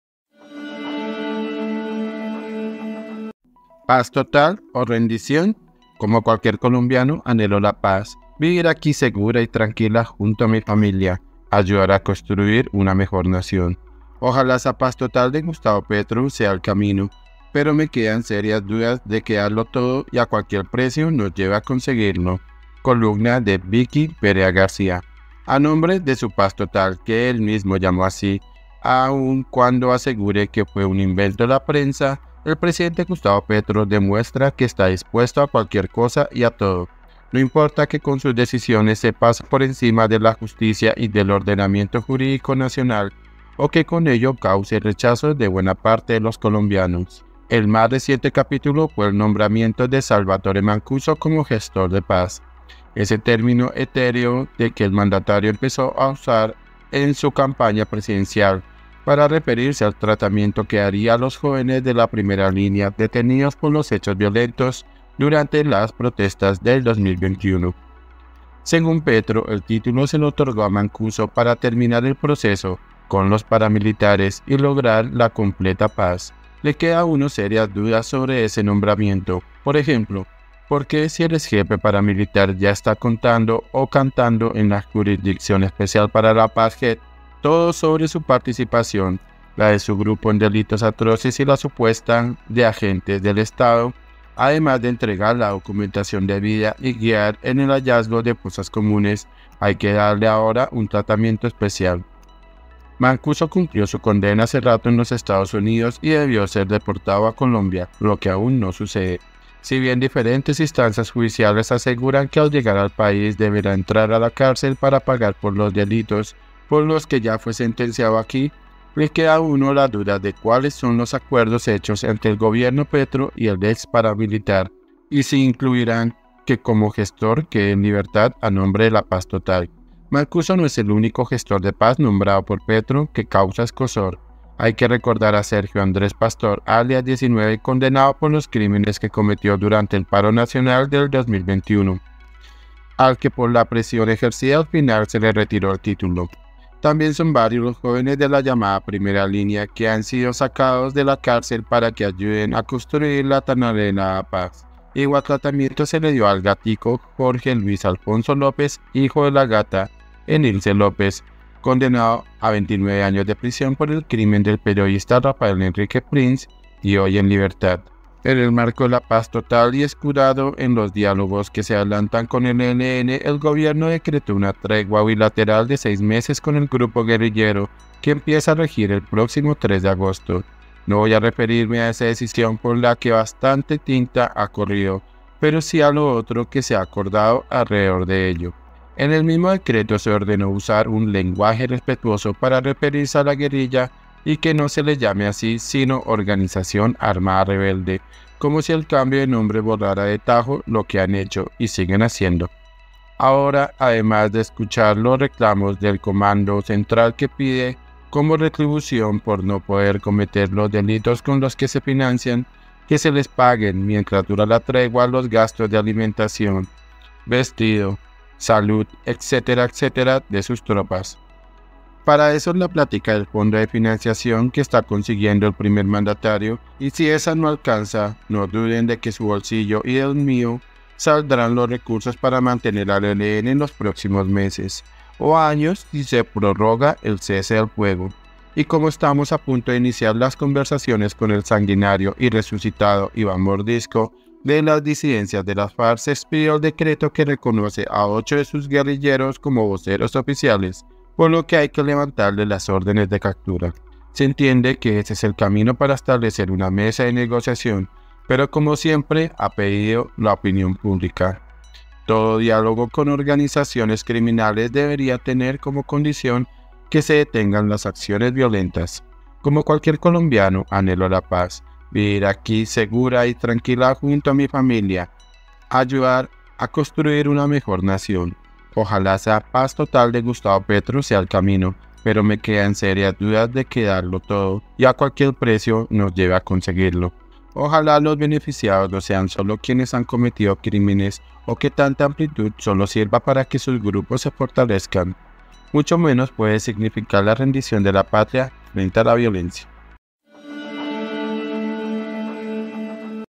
¿PAZ TOTAL O RENDICIÓN? 🚨 Columna Vicky Perea García
A nombre de su Paz Total, que él mismo llamó así aun cuando aseguré que fue un invento de la prensa, el presidente Gustavo Petro demuestra que está dispuesto a cualquier cosa y a todo. No importa que con sus decisiones se pase por encima de la Justicia y del ordenamiento jurídico nacional, o que con ello cause el rechazo de buena parte de los colombianos.
columna VICKY PEREZ GARCÍA